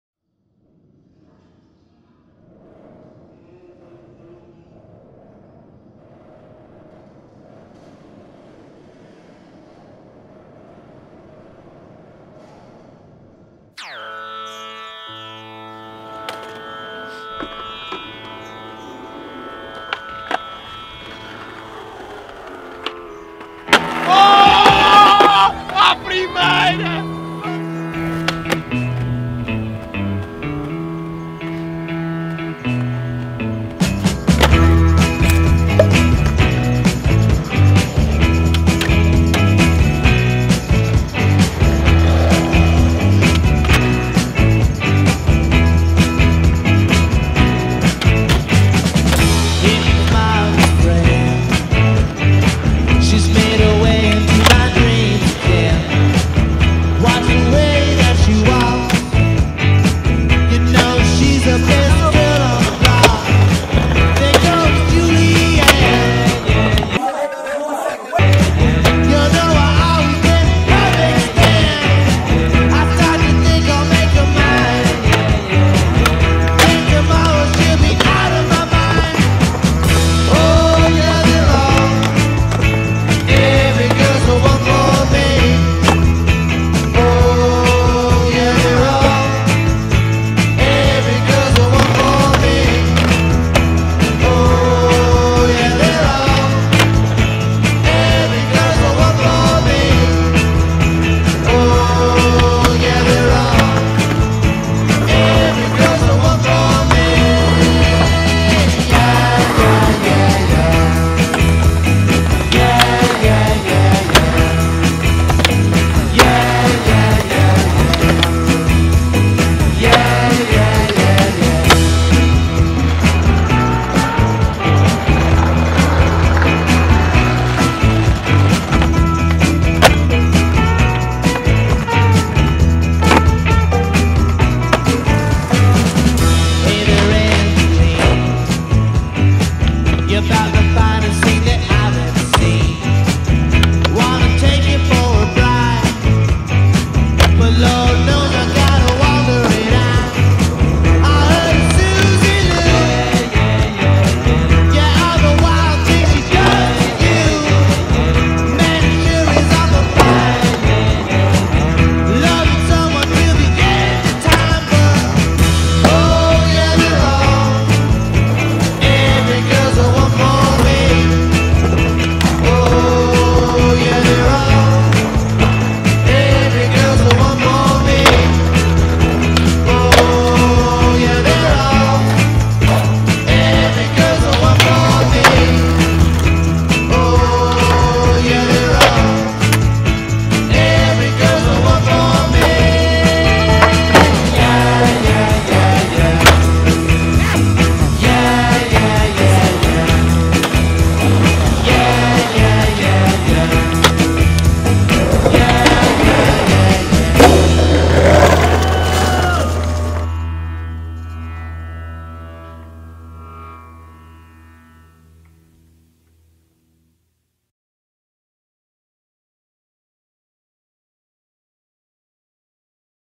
etnies Portugal - Street Wilson
Let's go Street Wilson!

Videopart with Gonçalo Vilardebó by Tomás Fernandes